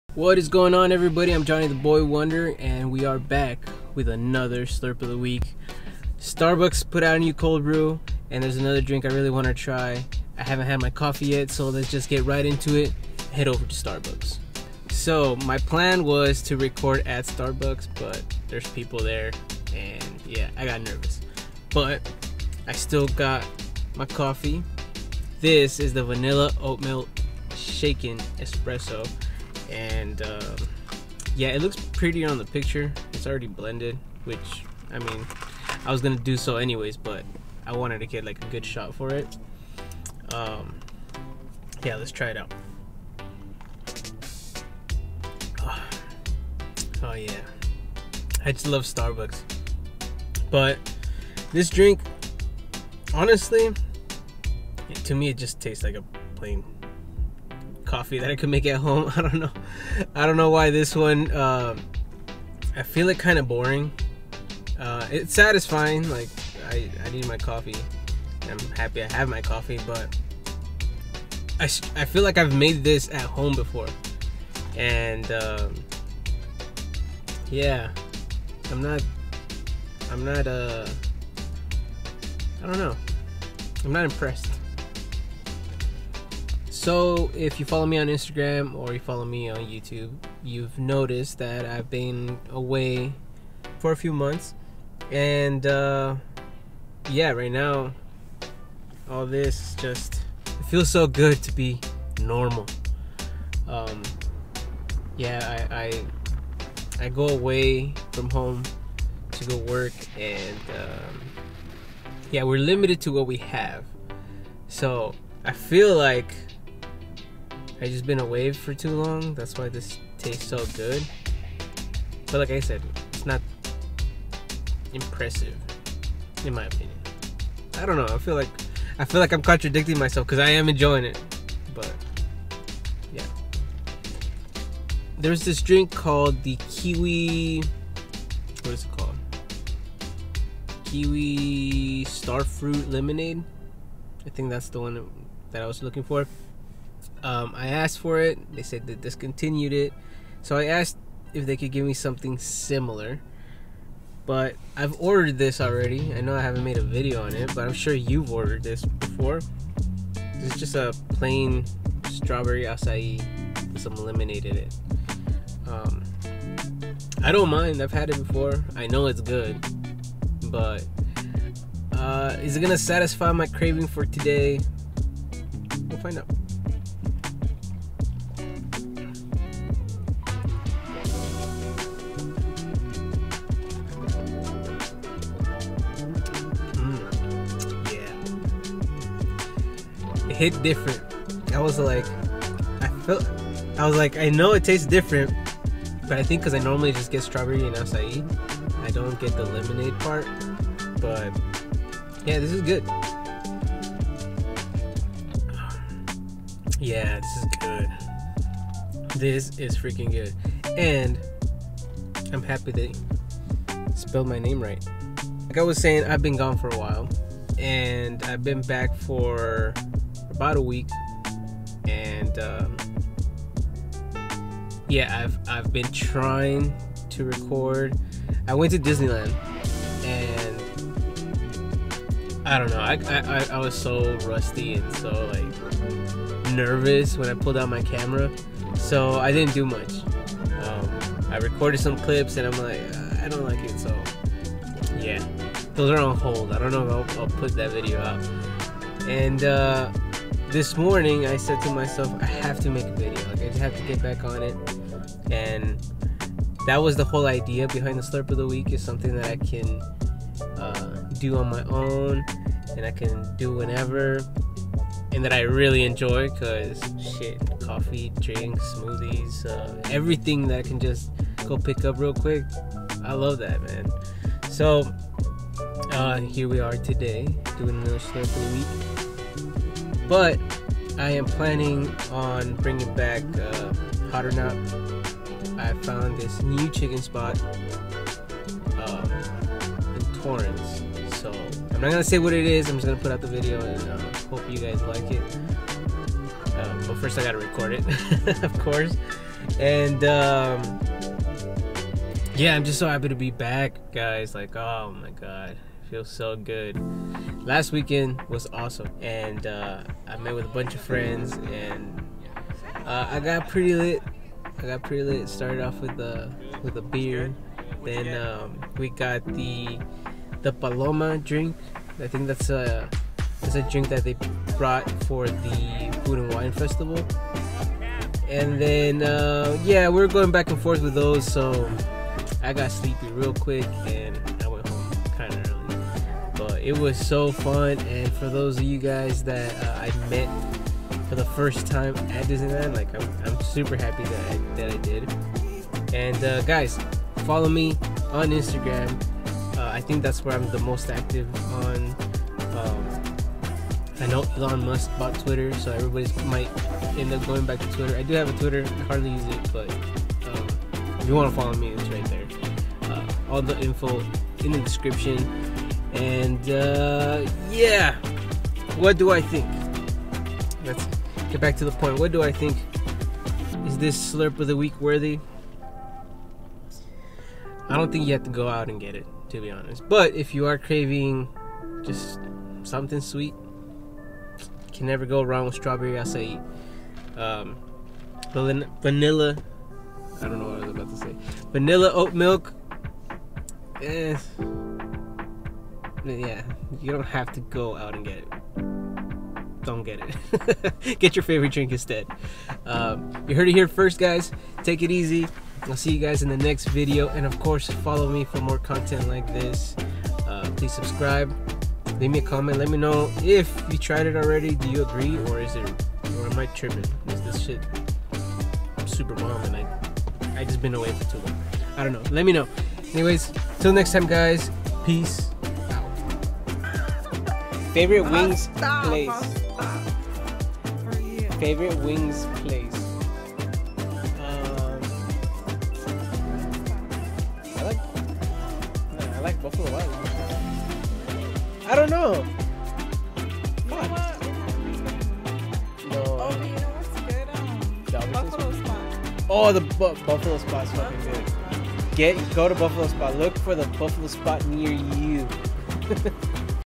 NEW Iced Toasted Vanilla Oatmilk Shaken Espresso From, Starbucks - JohnnyTheBoyWonder
Today I try out the NEW Iced Toasted Vanilla Oatmilk Shaken Espresso from Starbucks, and try to catch up with what ive been up to lately.